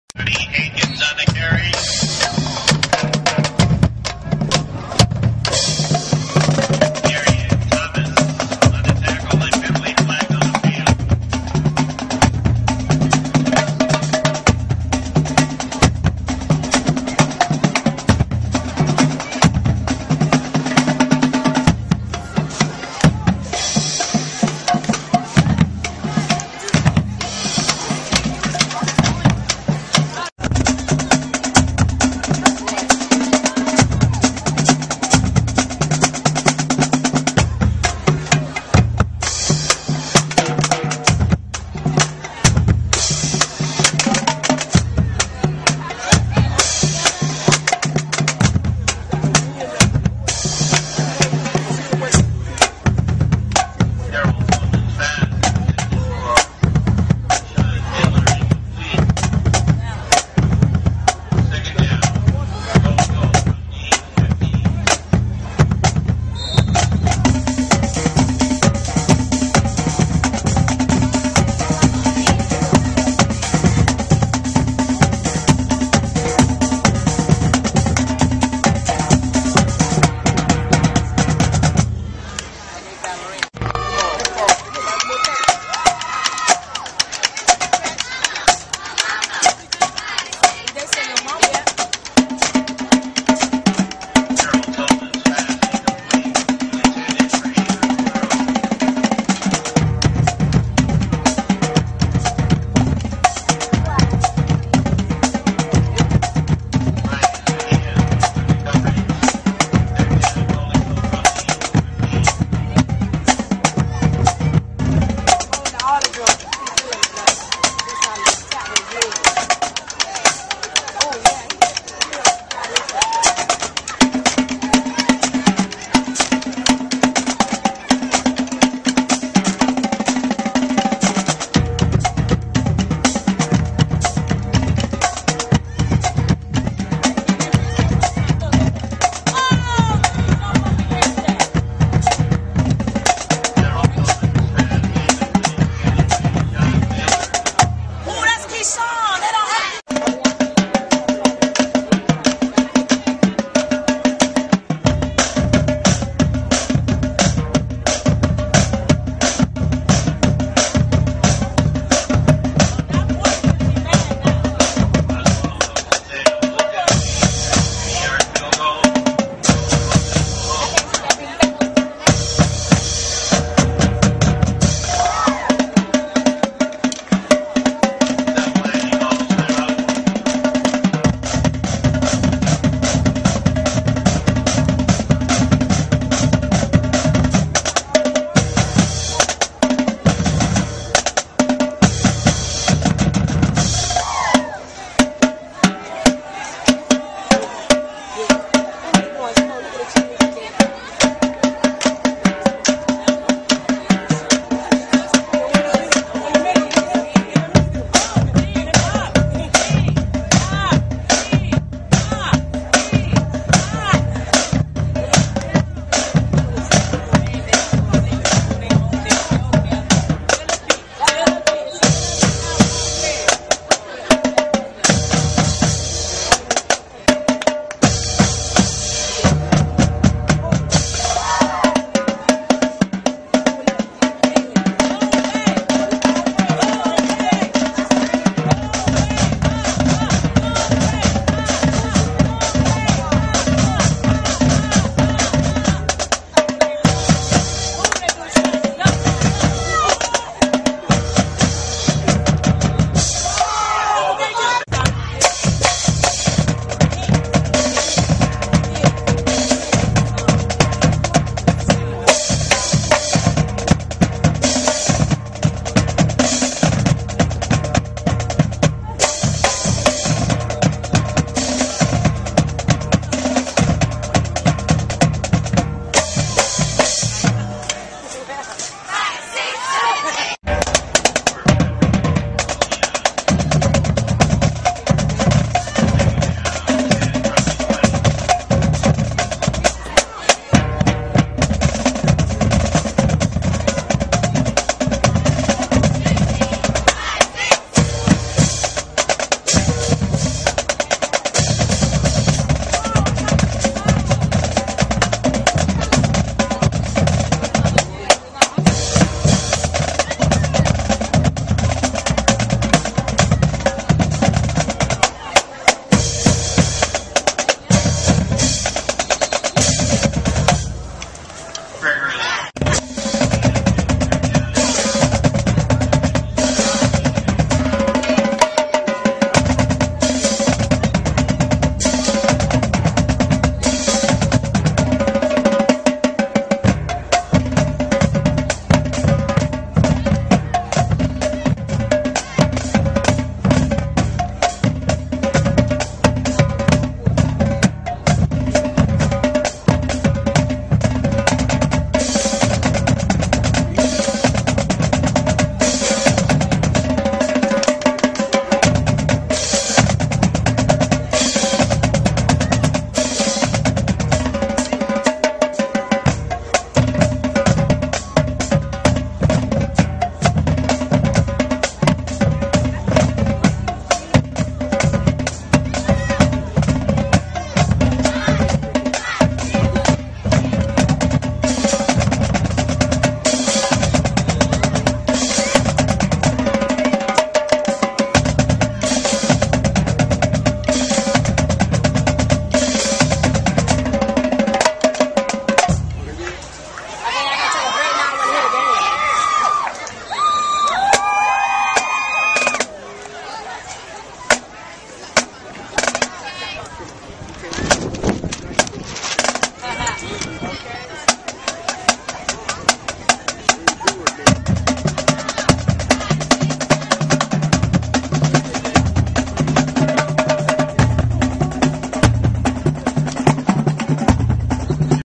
Band of Distinction Drumline 2011
B.o.D Cavalier Drumline playing for the Crowd at a football game.